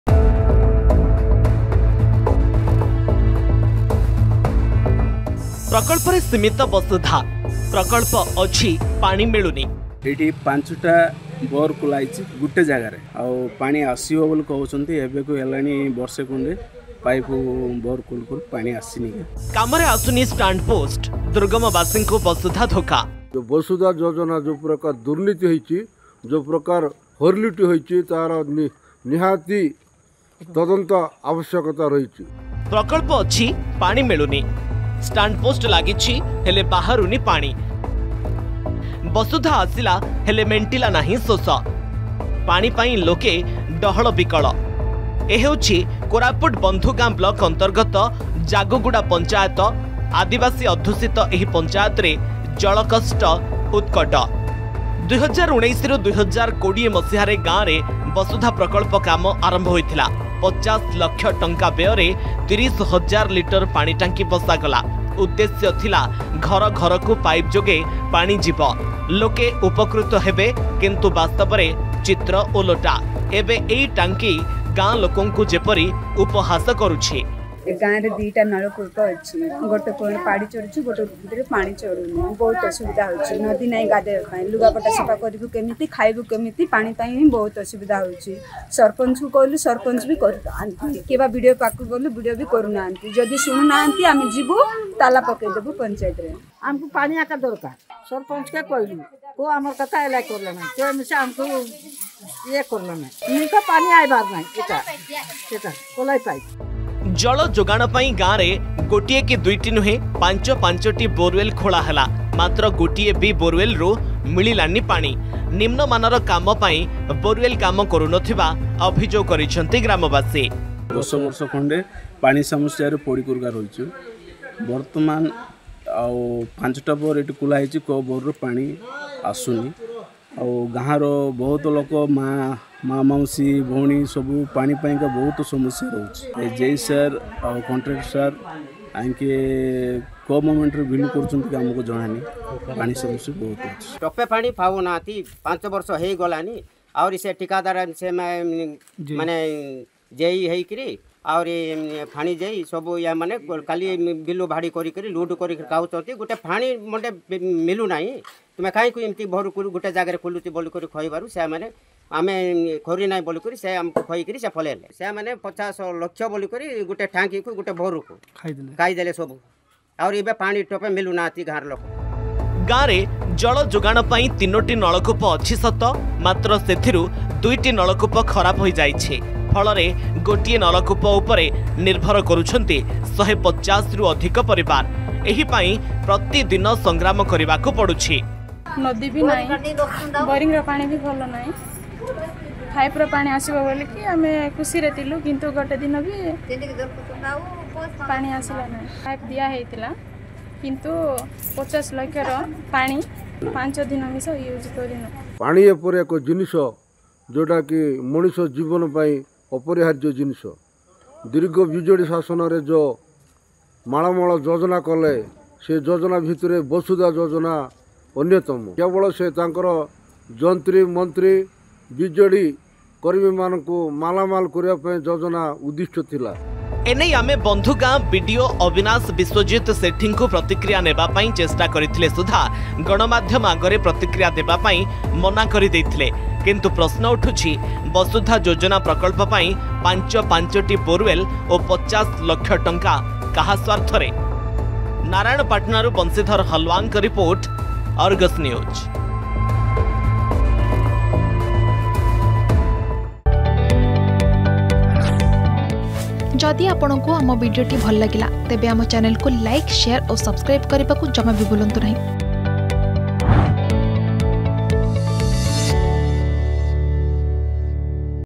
ପ୍ରକଳ୍ପରେ ସିମିତ ବସୁଧା | Basudha Project Fail in Koraput | Argus News
ପ୍ରକଳ୍ପରେ ସିମିତ ବସୁଧା, ପ୍ରକଳ୍ପ ଅଛି, ପାଣି ମିଳୁନି! କାମରେ ଆସୁନି ଷ୍ଟାଣ୍ଡପୋଷ୍ଟ। ଦୁର୍ଗମବାସୀଙ୍କୁ ବସୁଧା ଧୋକା, ଏ ହେଉଛି କୋରାପୁଟ ବନ୍ଧୁଗାଁ ବ୍ଲକ୍ ଅନ୍ତର୍ଗତ ଜାଗୁଗୁଡା ପଞ୍ଚାୟତ। ଆଦିବାସୀ ଅଧୁସିତ ଏହି ପଞ୍ଚାୟରେ ଜଳକଷ୍ଟ ଉତ୍କଟତର।